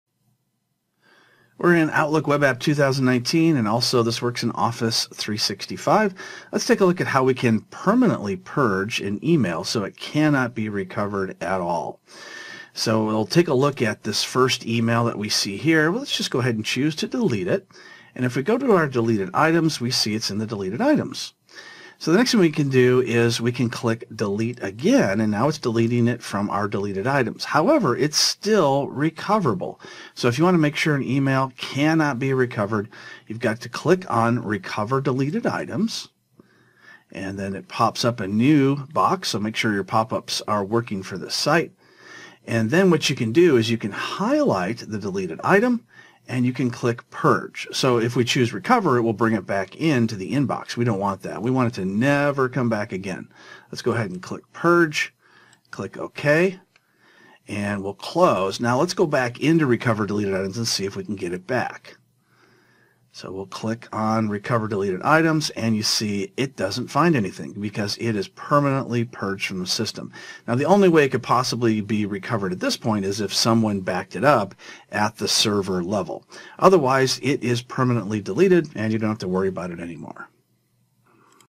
How to permanently delete an email using Outlook Web App (OWA) 2019
Professor Robert McMillen shows you how to permanently delete an email using Outlook Web App 2019